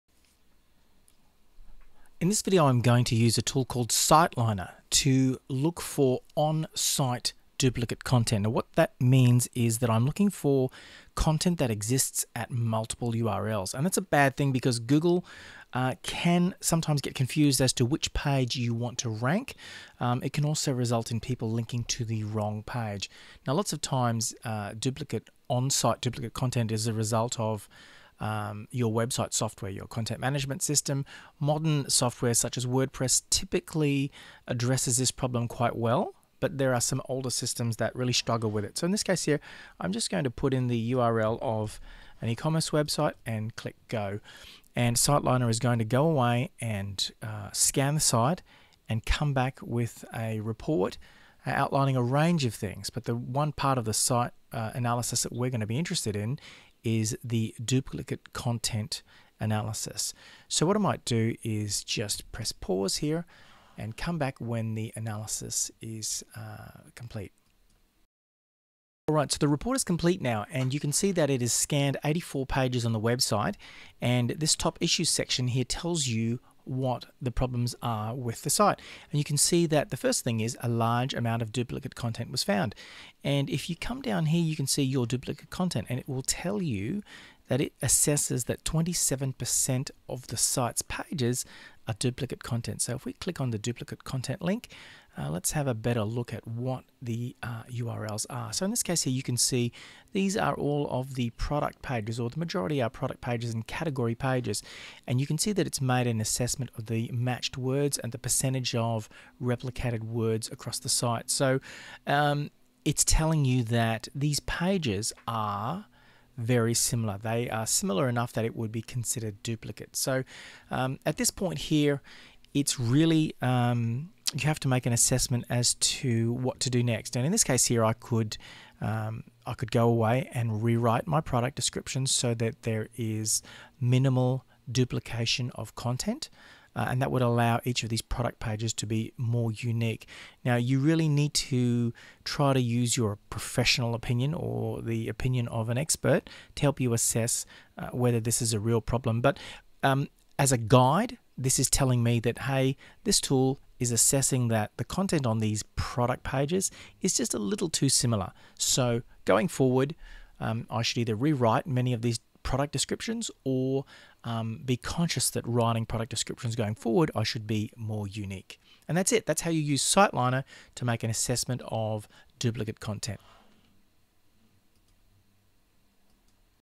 How to Check for Duplicate Content on YOUR website
Having content on your site duplicated across many pages and URL's is a sign of low quality and can impact how your website performs in search engines.  In this video I show you how to use a free tool to quickly check your site for excessive onsite duplicate content